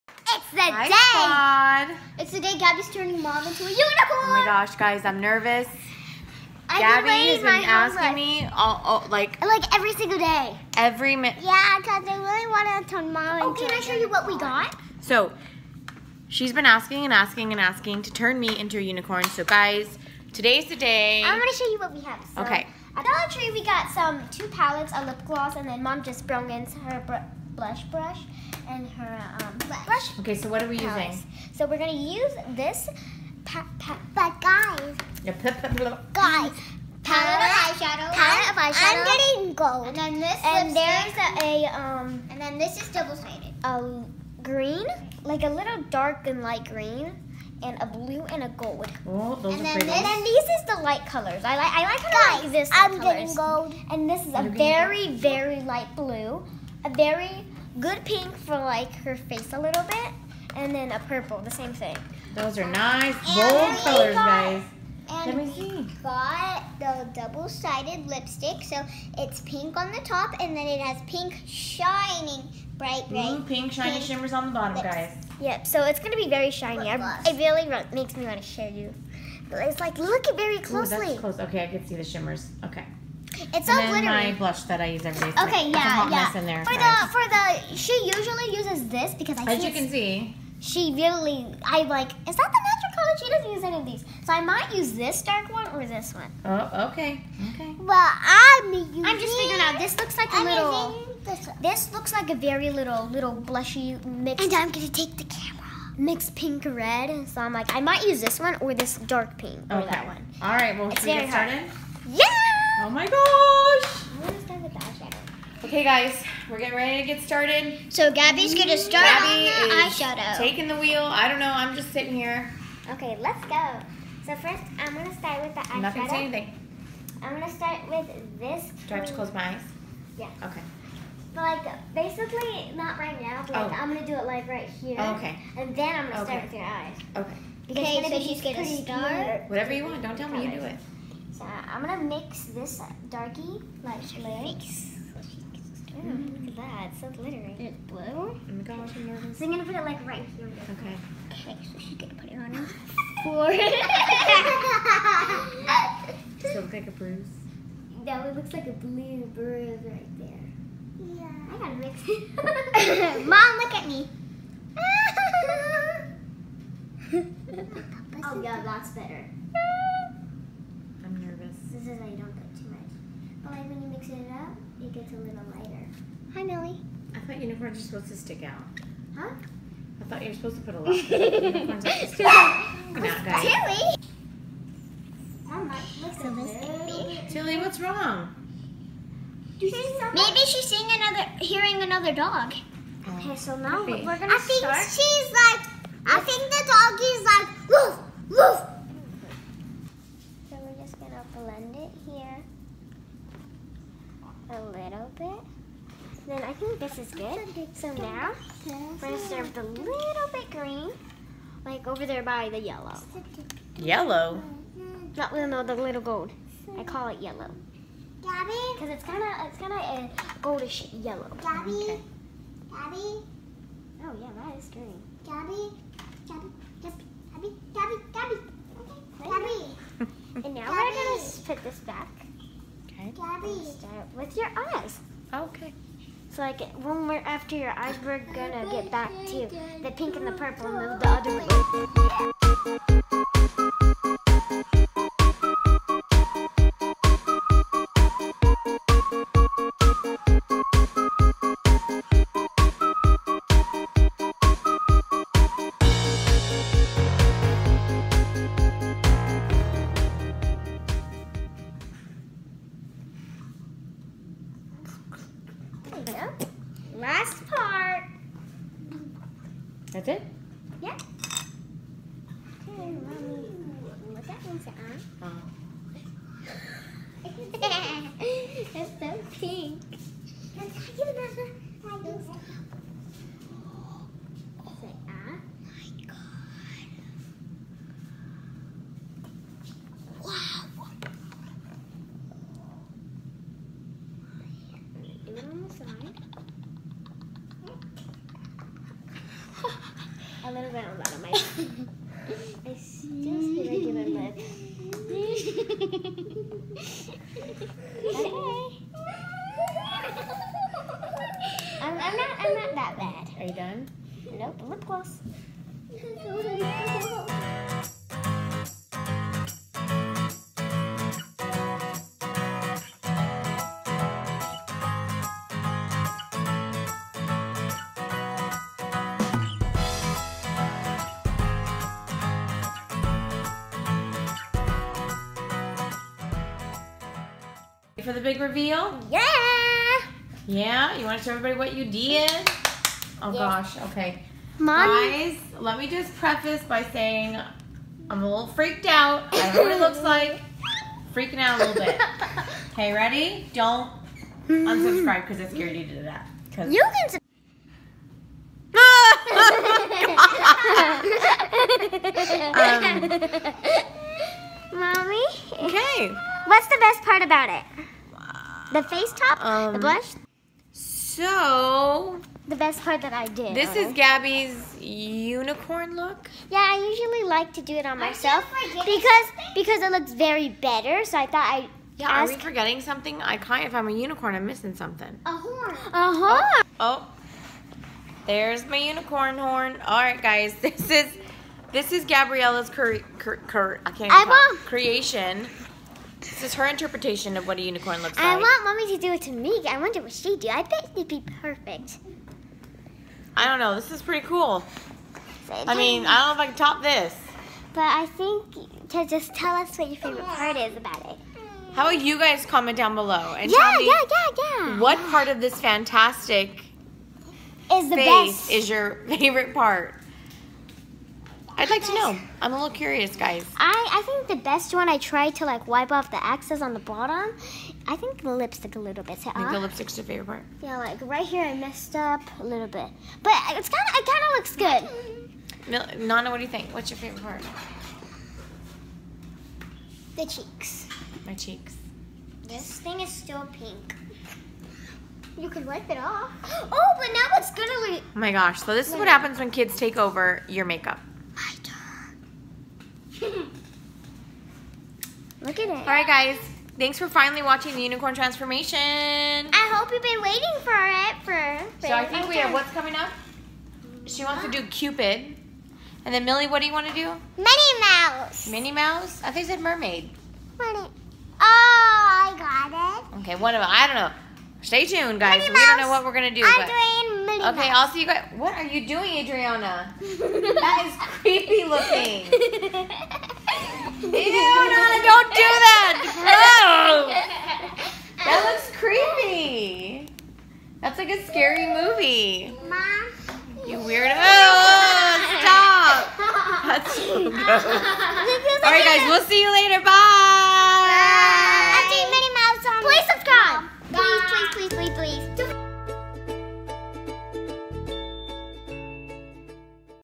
KIDS TURN MOM INTO UNICORN - KIDS TAKE OVER - MAKEUP MAKEOVER - FAMtastic5
Hey Squad! So Gabby has been bugging me to transform me into her version of a unicorn. Since she is obsessed with makeup I finally allowed it. Man oh man was I surprised at how fabulous I looked. You won’t want to miss this fantastic transformation!